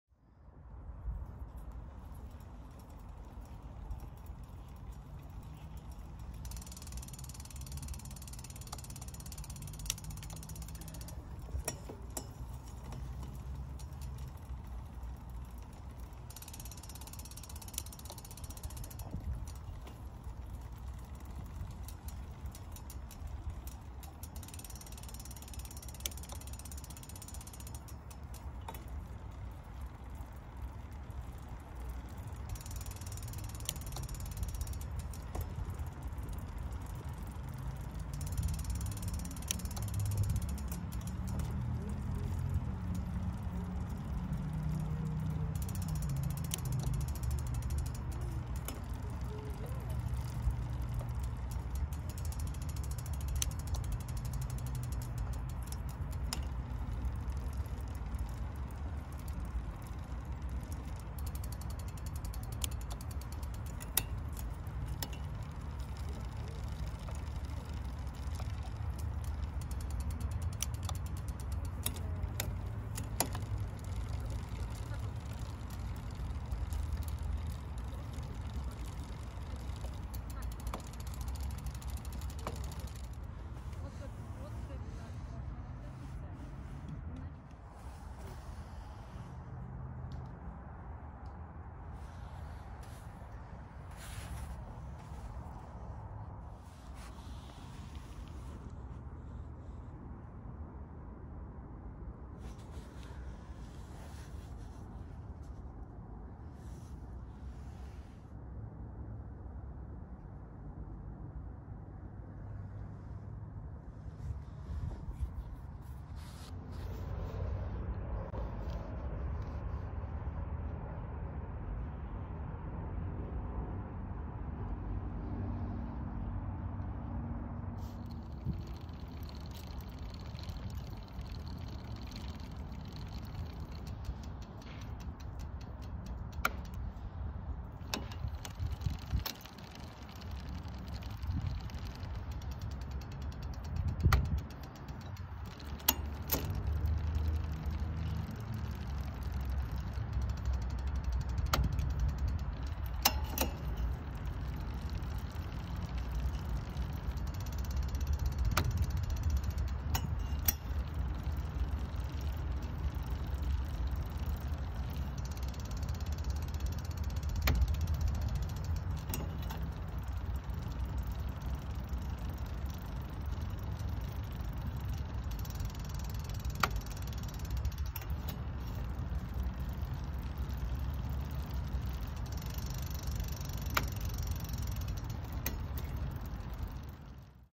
Shimano ZEE and 11 Speed (11-46t) cassette !!! it works perfect
Haha, I just noticed the movie starts in second gear, of course they all work.  Error while editing the movie :)